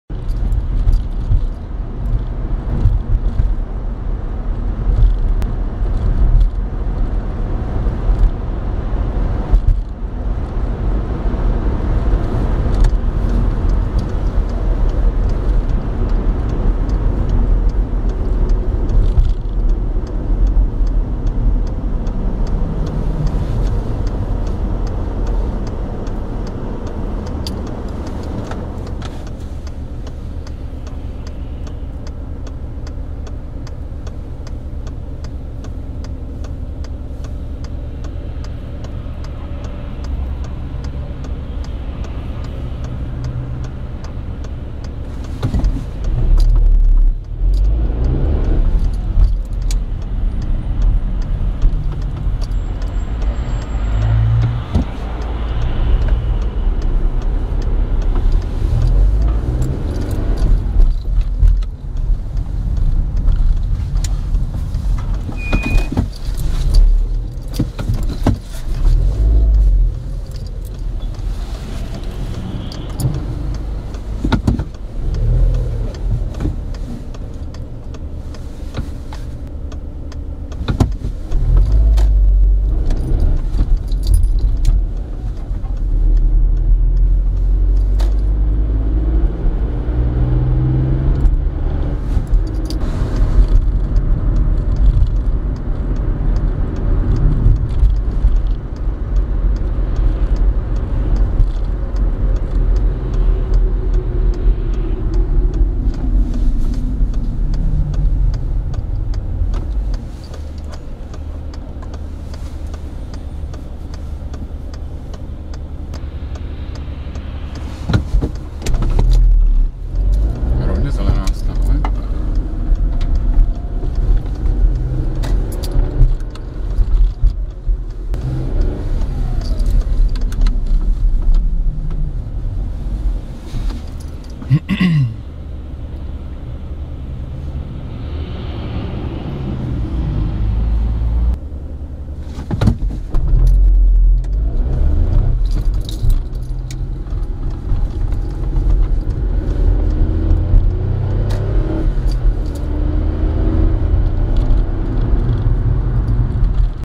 SSZ Vídeňská -- U krčského nádraží 1.12.2013
prostříhaný videoprůjezd ze všech směrů, ale vždy vlevo :)